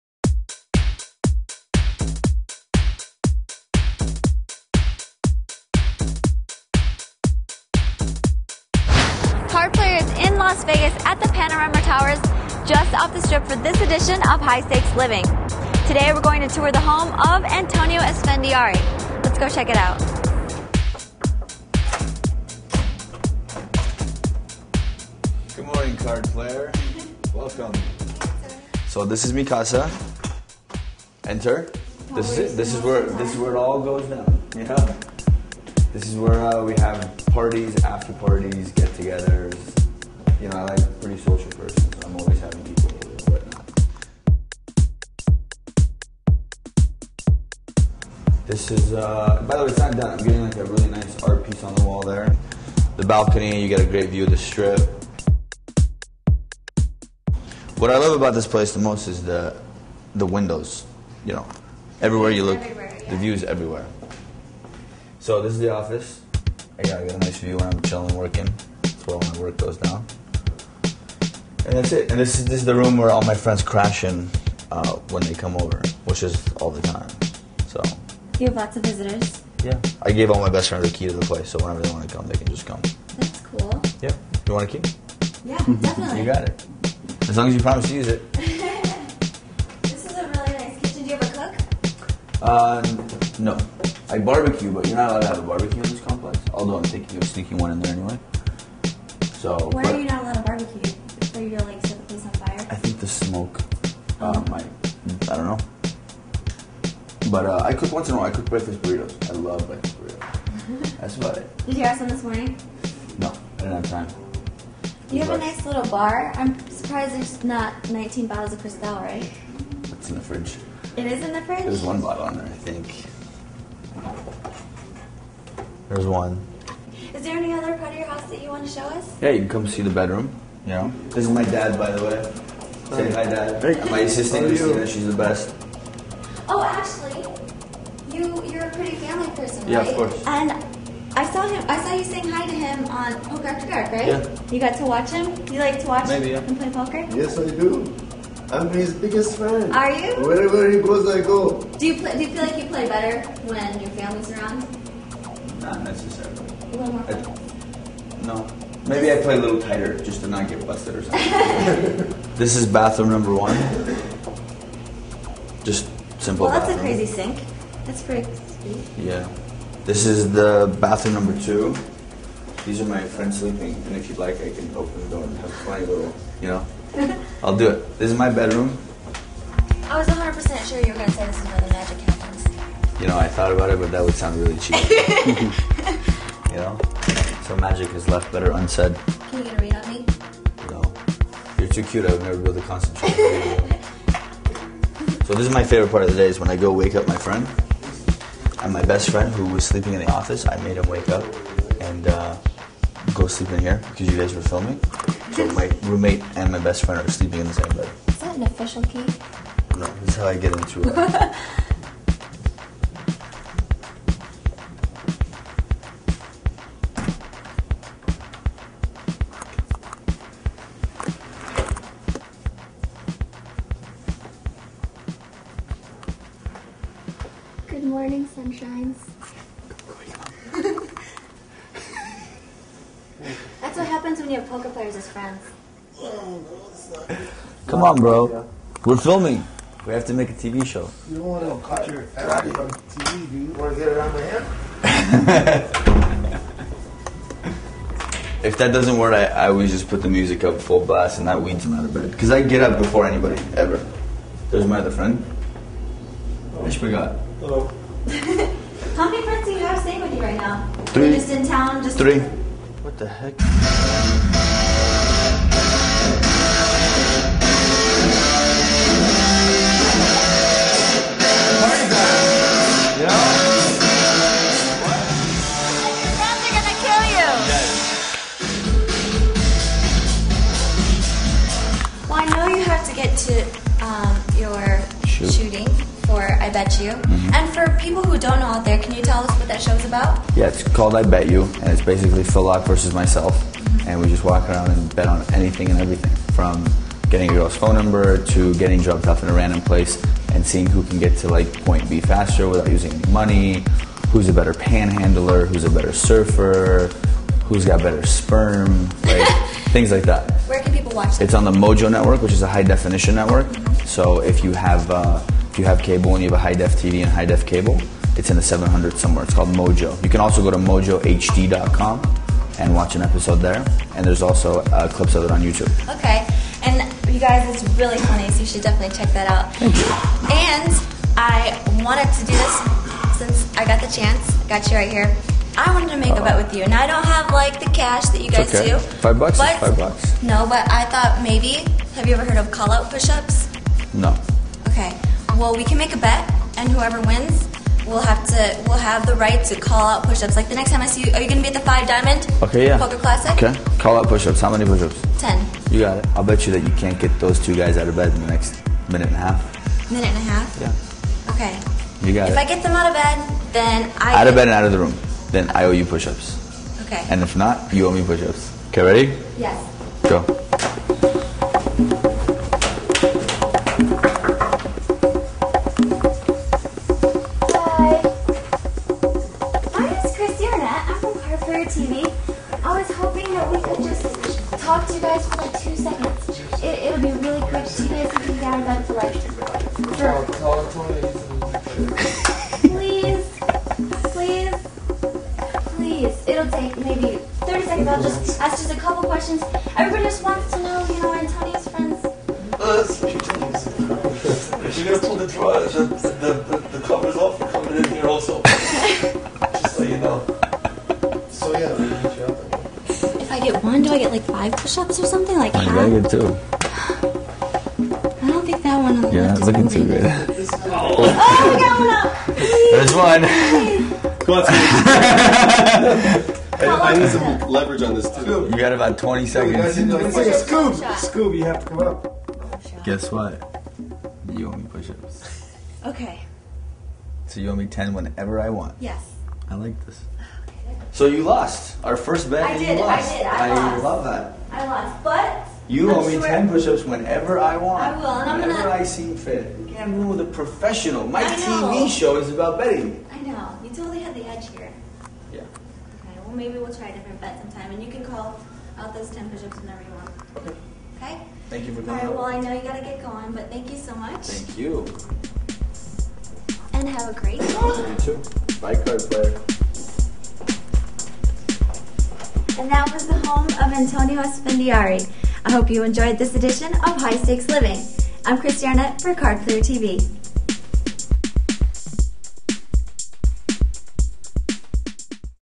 High Stakes Living: Antonio Esfandiari
Card Player TV takes an exclusive tour of the home of Antonio Esfandiari.